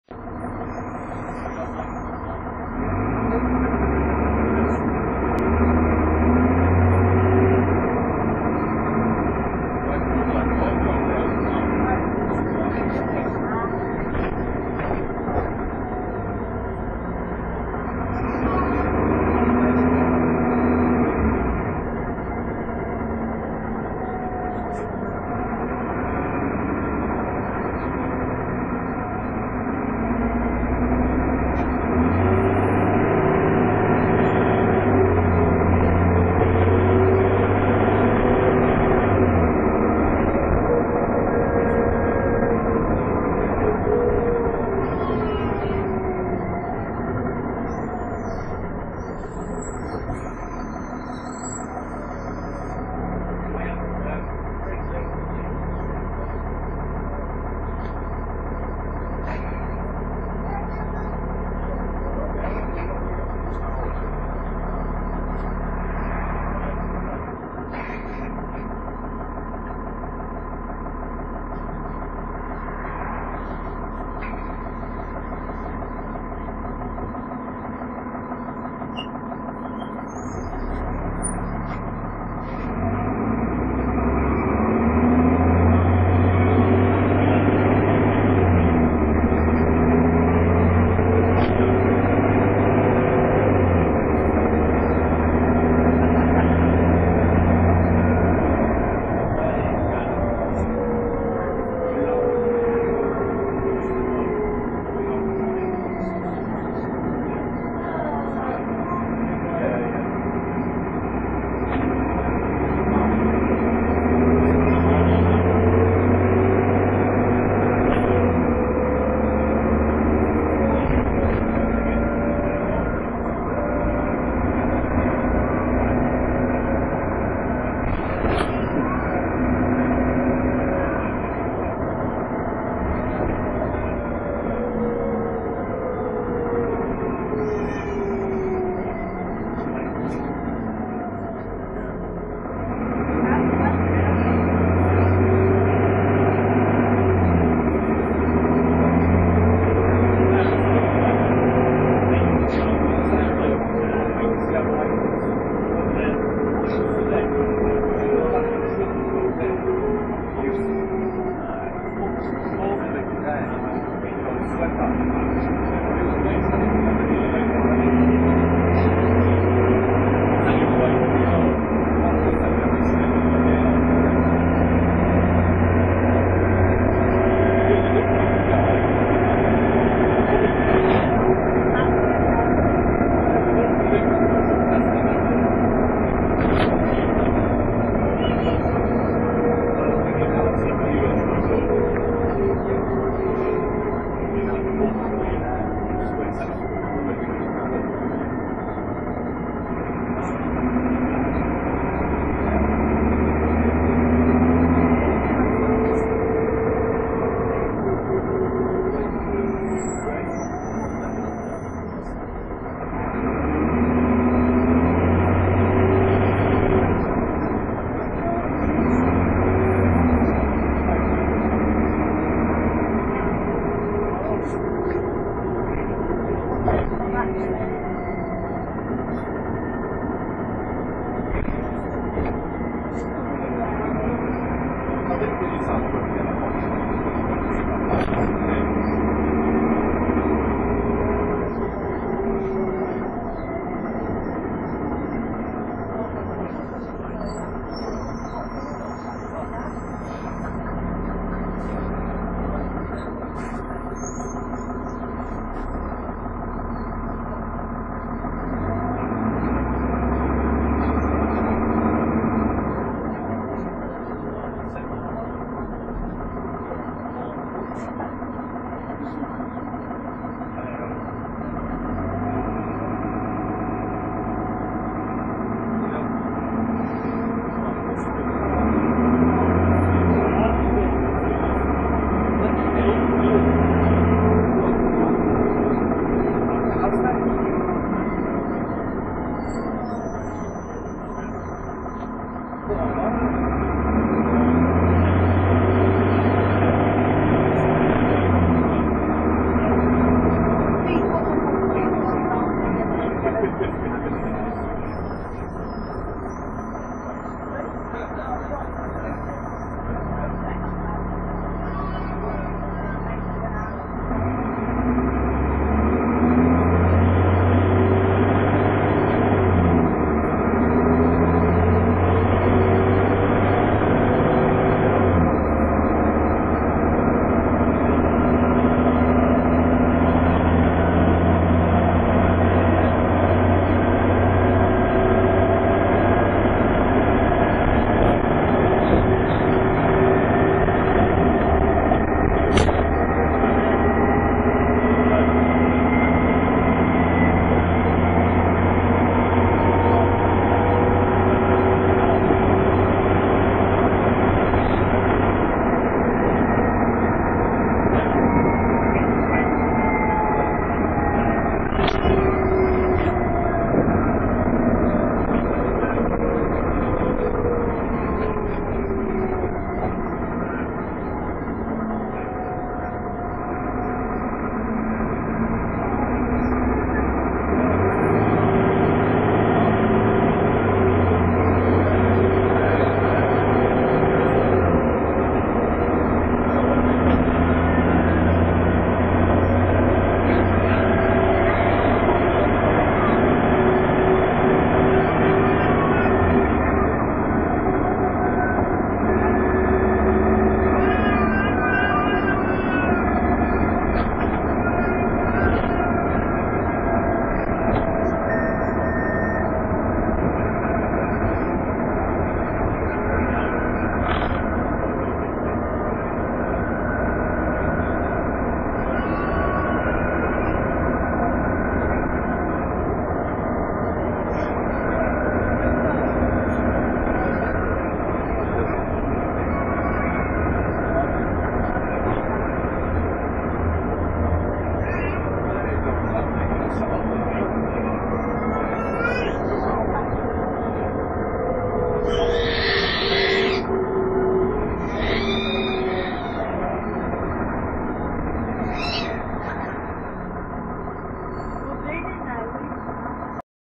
[THRASH] Stagecoach Hull 18432 YN06LMJ on 15 to Interchange 20141101 Part 3
Stagecoach Hull Dennis Trident 2 Alexander ALX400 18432 YN06LMJ with massive kickdown and thrash on route 15 towards Hull Interchange on 1st November 2014 (Part 3 of 3). This is the video from the second trip on this bus today.